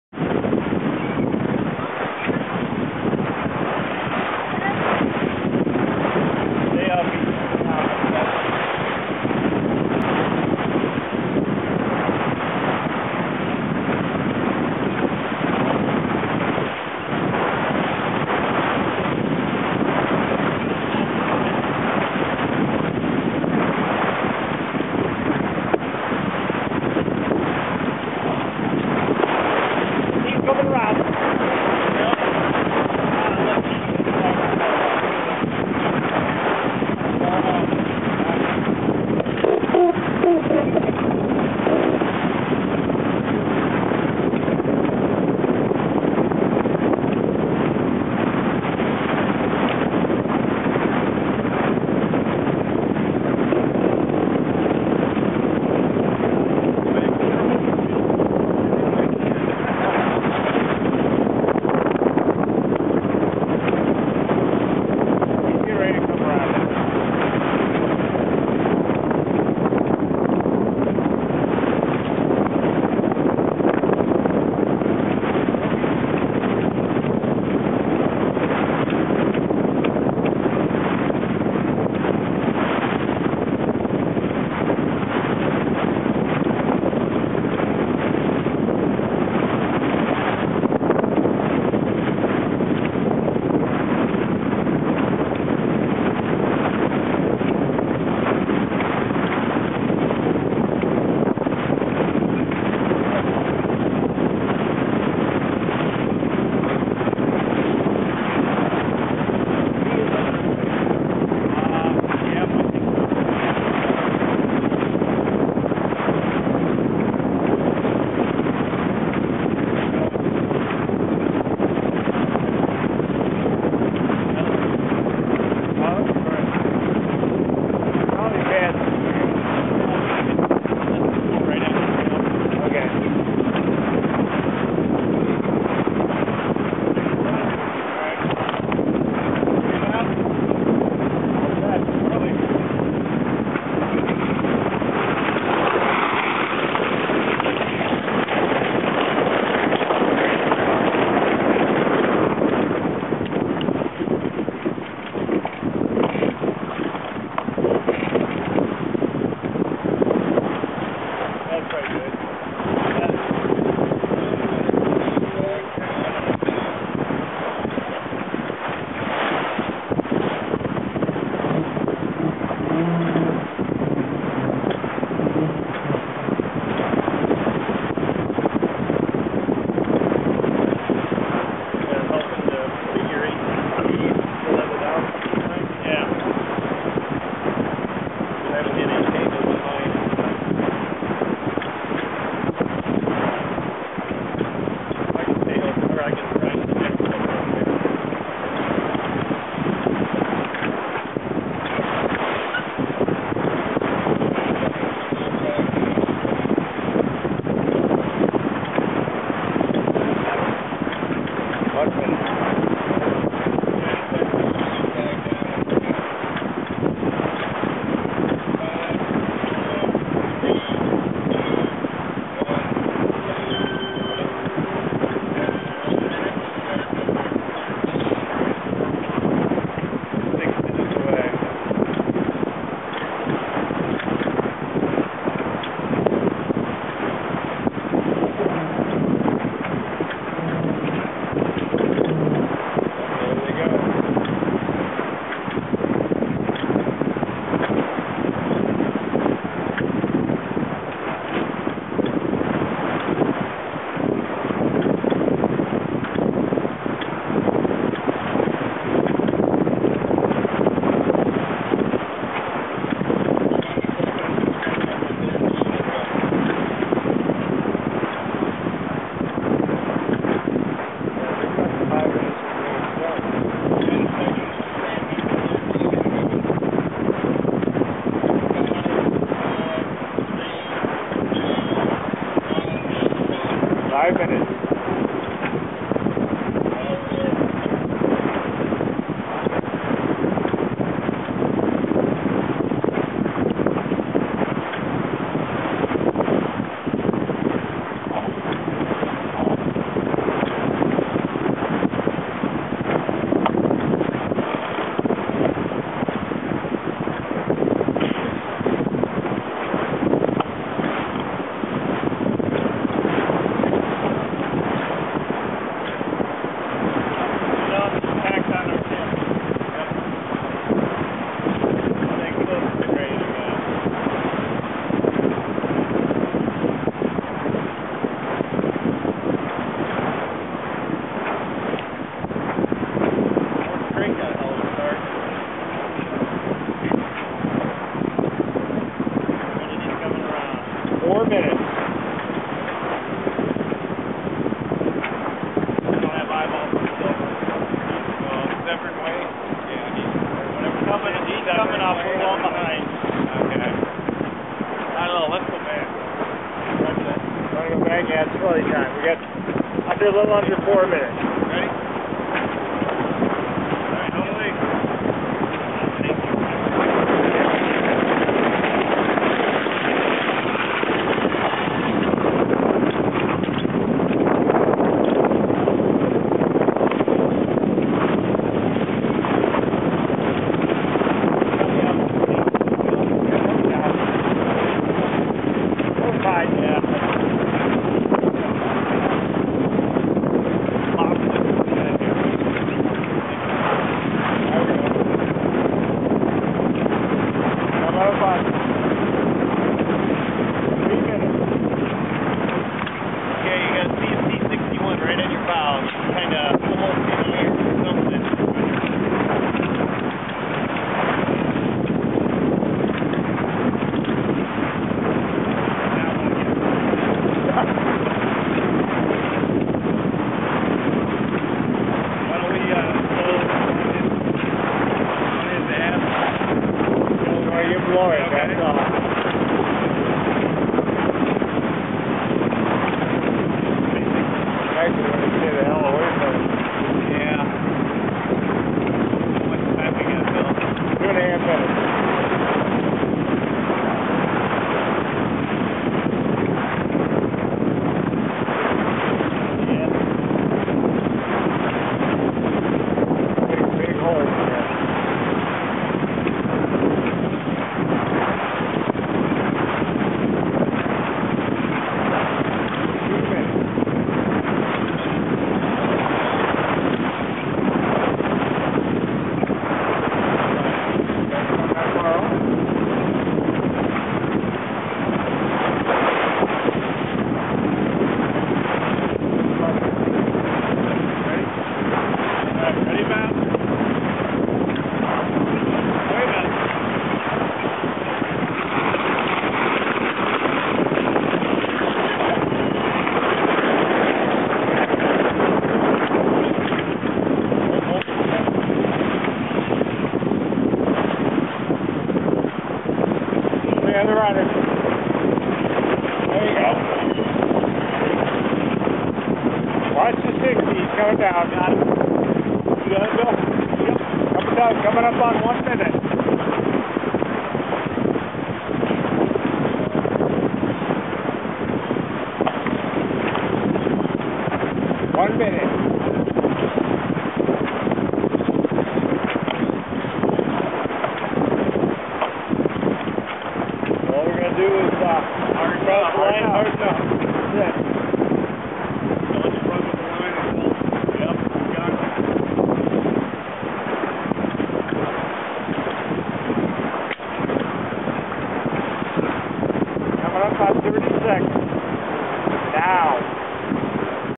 Mills Trophy Pre Race Start 2010
Just a couple of minutes of Pre Race Start from the Mills trophy race 2010.  Looking back